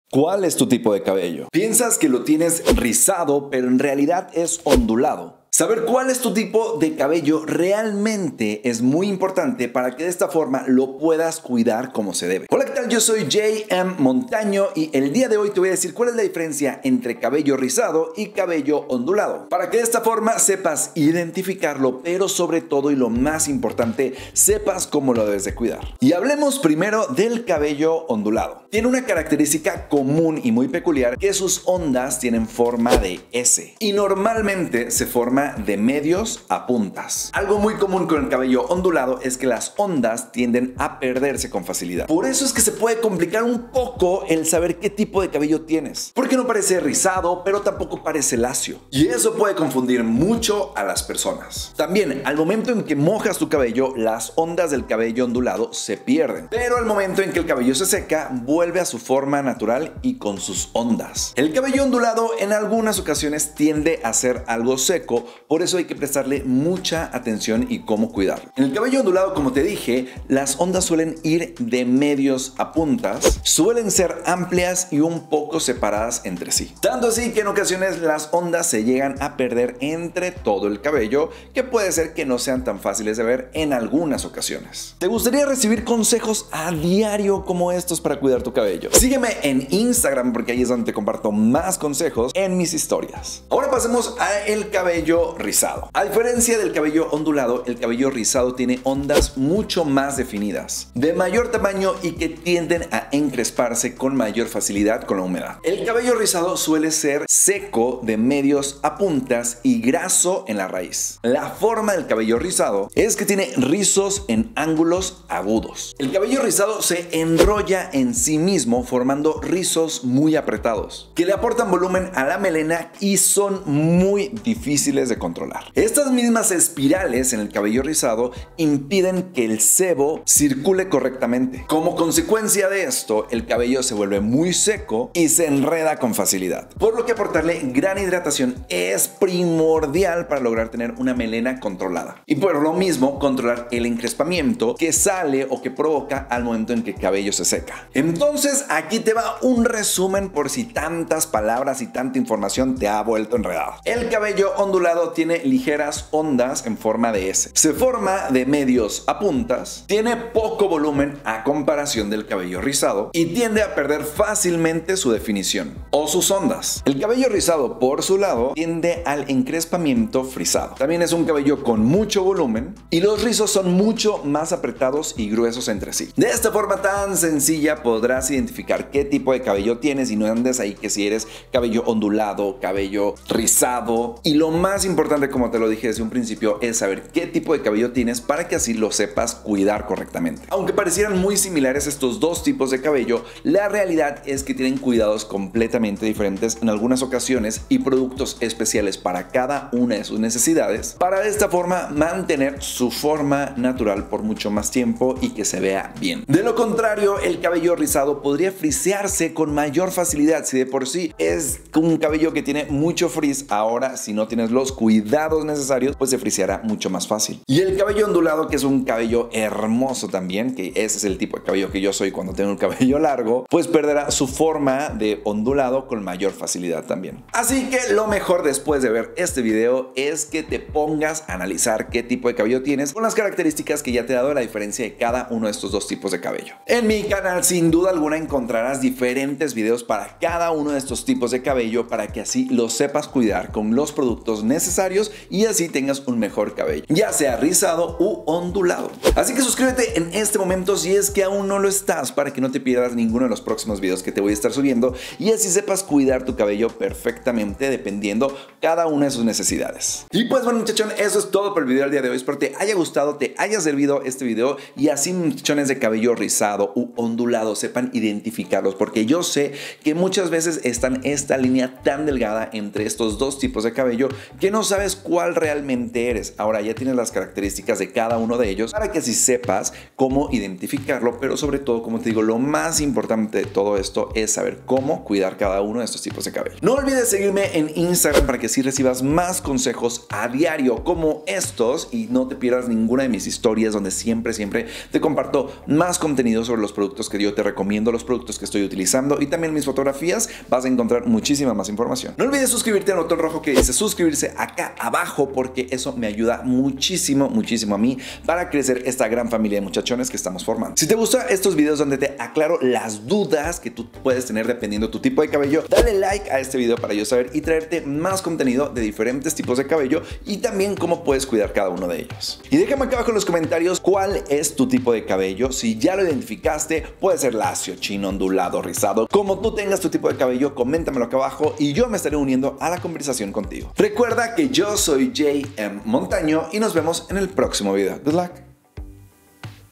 DIFERENCIA DE CABELLO RIZADO Y ONDULADO - J.M. Montaño
Las diferencias entre el cabello ondulado y rizado pueden ser mínimas , pero muy importantes al momento del cuidado y peinado de ambos cabellos.
Por eso hoy te explico las diferencias que existen entre estos dos tipos de cabello para que sepas aprovechar sus beneficios al máximo y lo cuides como se debe.